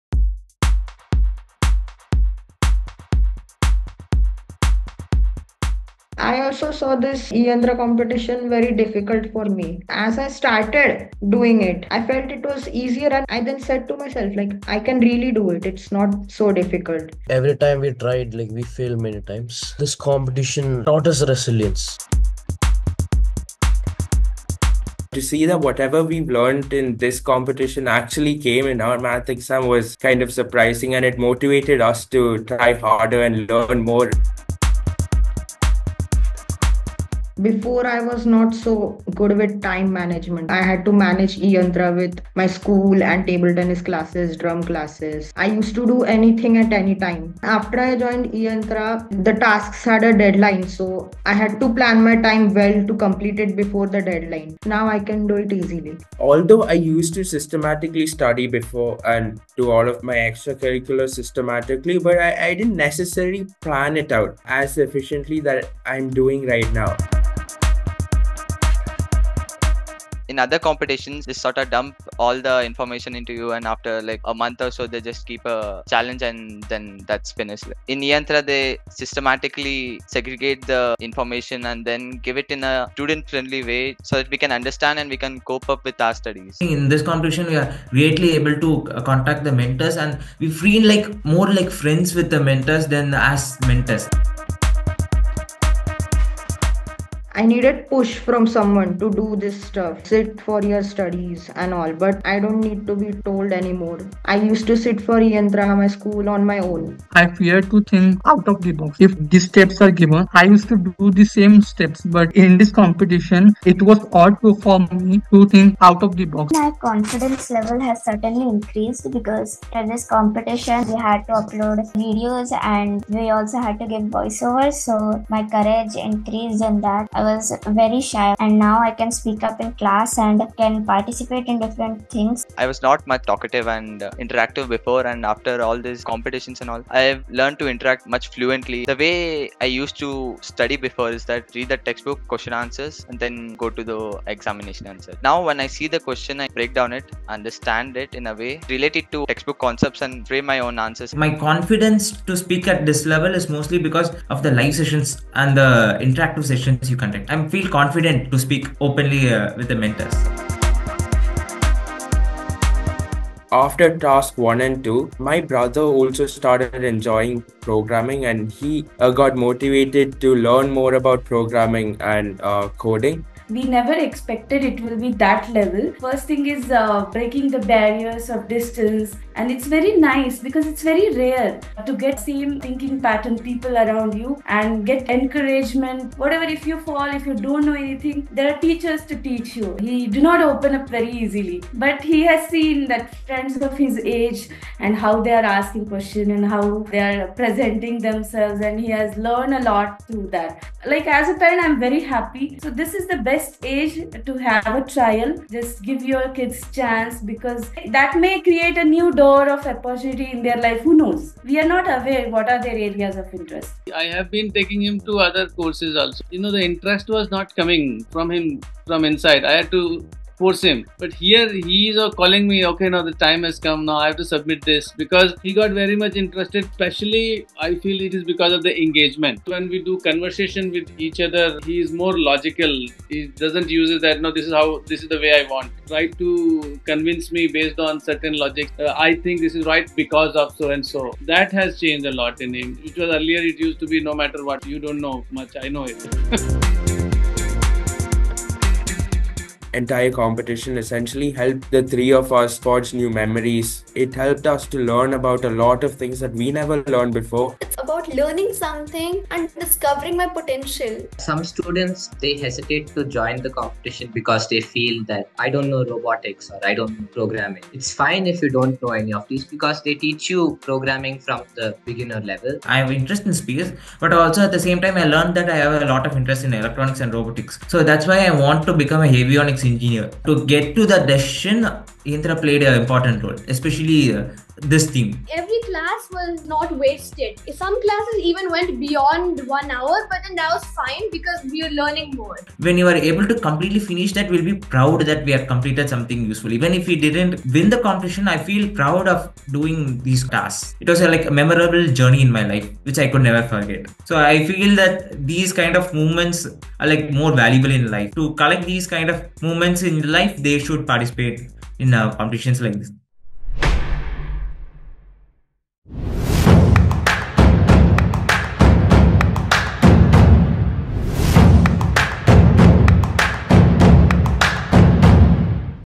Intrepid Explorer Edition 2 - Student Impressions Film 2025
e-Yantra school robotics competition is designed for anyone to start from scratch and learn programming. Infact, apart from technical skills, you learn precious life skills which are critical in honing your overall personality. Students develop resilience through many trials. Some of the students believe that what they learnt in eYSRC had an impact on their academics. Students also learn to manage their time effectively during the course of this competition. In eYSRC you get learning resources & there are doubt clearing sessions online. Focus is on learning & not to just run a competition. e-Yantra Mentors provide adequate guidance during the course of the competition. Students learn to take responsibility of each and every task which is a big shift from haggling them to study. They also learn to explore various possibilities to solve a problem. One can see a huge change in confidence level during the course of the competition. Some students shared that their sibling also got interested in programming because of eYSRC and some parents shared about how unique it is to find students from across the country inclined towards learning something new. They think that other parents should let their child explore this competition as it may open up some new doors for them. Parents also feel that their child has more logical arguments than before. Students discover their immense inner potential trough this competition. Students are encouraged to participate in a team in this competition so that they can not only learn teamwork but also forge friendship and create new memories together. Students will feel proud of themselves for completing the task at each level. Students feel these moments are more valuable in life!

Key Points –
00:00 Learnt the spirit of challenging my limitations
00:21 Impact of eYSRC on school academics
00:35 Learnt to manage my time effectively
01:08 Difference between eYSRC & other robotics competition
01:35 Transformation after participation
02:41 Impressions of family members
04:07 Why participate in eYSRC?